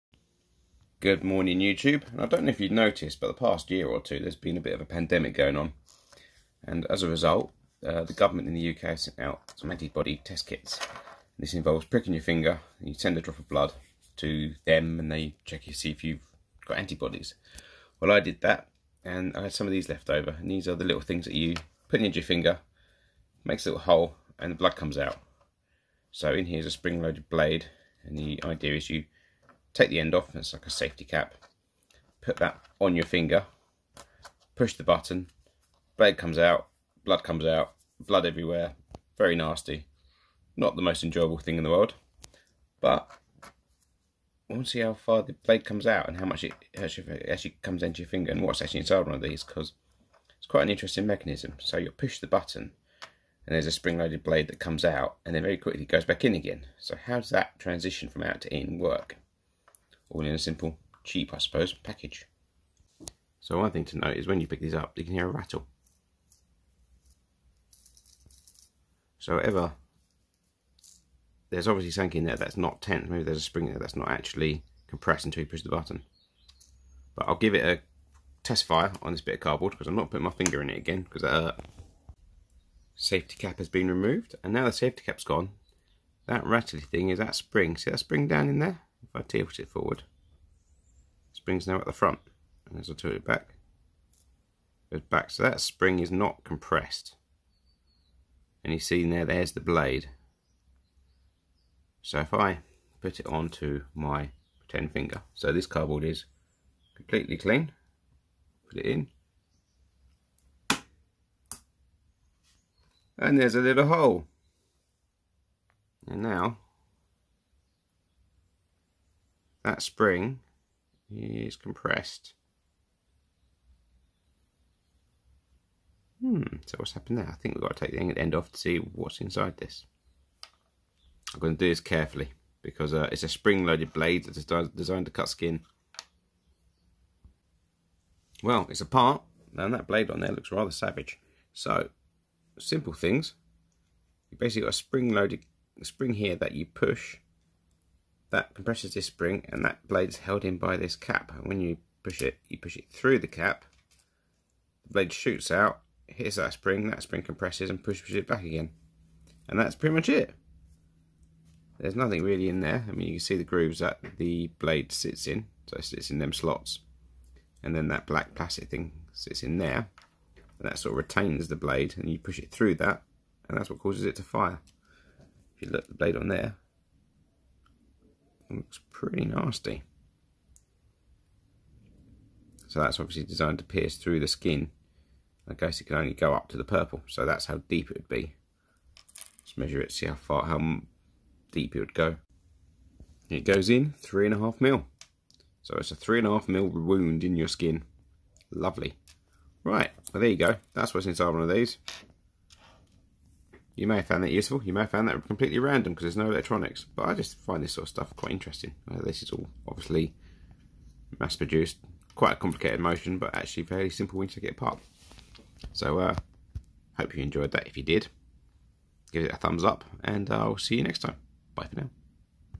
finger stabbing device of pain and blood
owch, look an device disigned to get blood.

BTC - 3A8oQxxR1di4GeWWEuJ4J42Zm7bWCRjwi8
ETH - 0xa499f8307F20478487B03a3a6945F5e0cc10e6cf